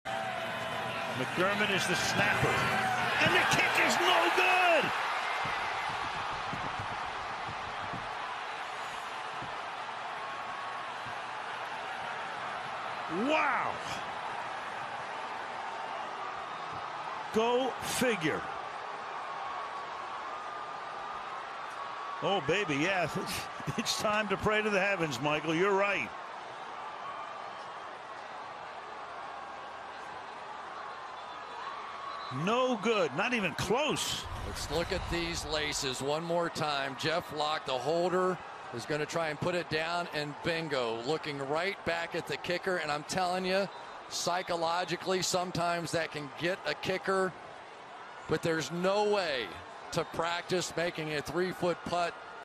Not Even Close! Blair Walsh Misses Game-Winning FG! | Seahawks vs. Vikings | NFL
Minnesota Vikings K Blair Walsh misses game-winning FG in NFC Wild Card Playoff Match-up with the Seattle Seahawks.